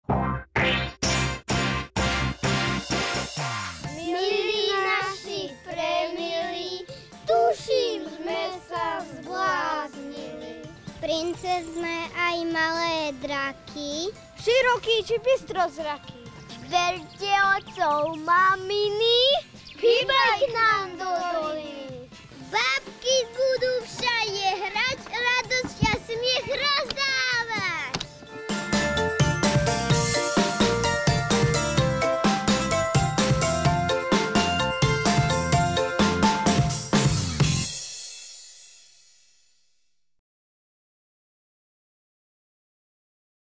ZA 7 HORAMi – detská pozvánka
Najväčší rozprávkový festival v prírode začne už 4. júla a tri dni bude baviť rodiny s deťmi. V 5 kráľovstvách zábavy v Liptovskom Jáne a Jánskej doline vystúpi až 5 divadiel s rôznymi divadelnými predstaveniami. Okrem toho na všetkých čaká skvelý doplnkový program plný zábavy, športu, tvorivosti a pravej prázdninovej nálady...

„Bábky budú všade hrať, radosť a smiech rozdávať... Deti, berte otcov, maminy a hybaj k nám do doliny.“ 😊 Viac o festivale ZA 7 HORAMi na